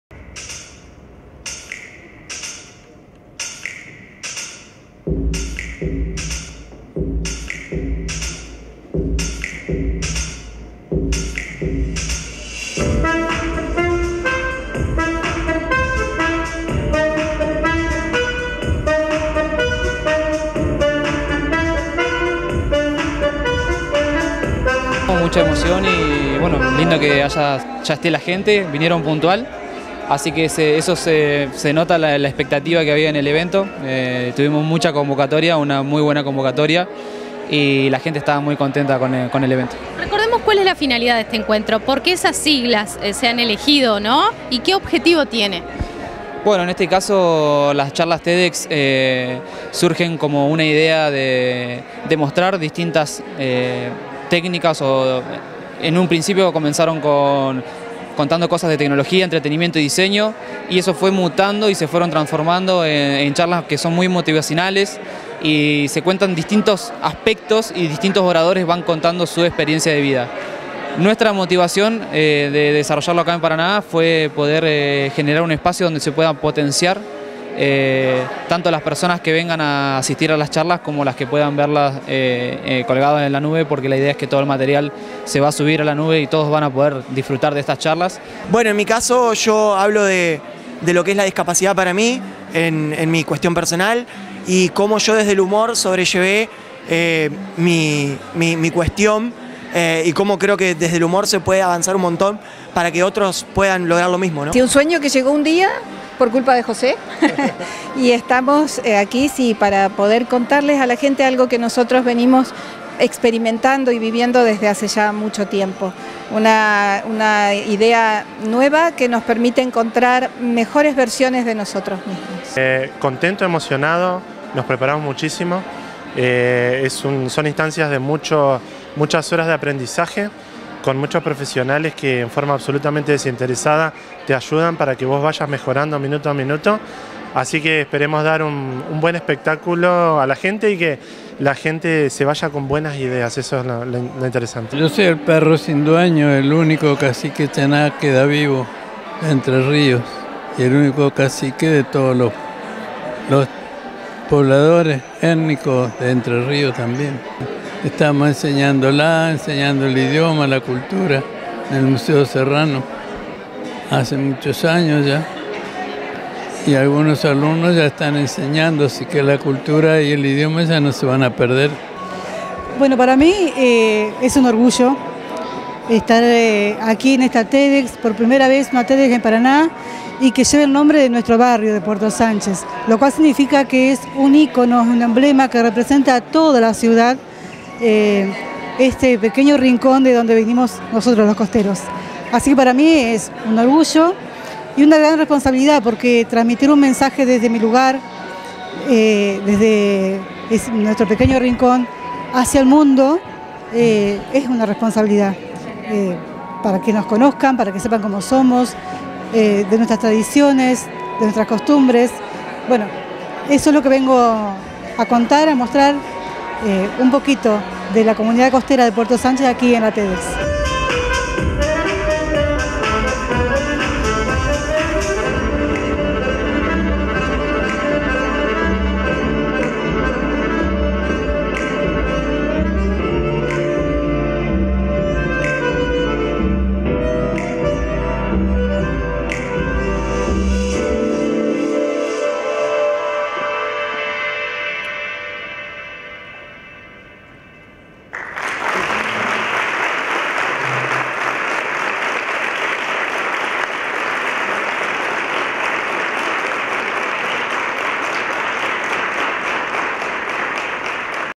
TED X PUERTO SANCHEZ
Por primera vez en Paraná se realizó en el Centro Provincial de Convenciones